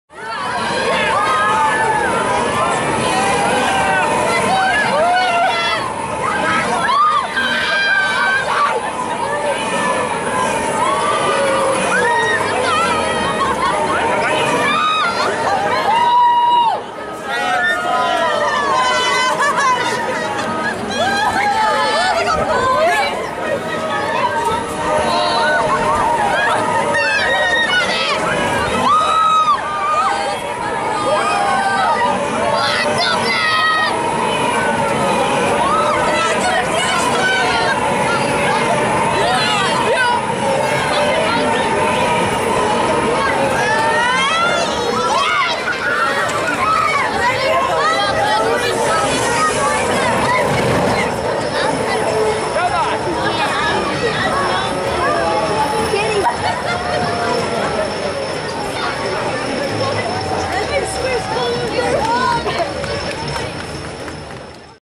Air Race - Emerald Park
Off ride footage of the Zamperla Air Race located at Emerald Park in Ashbourne, Ireland. The ride opened in 2015 alongside many other new attractions.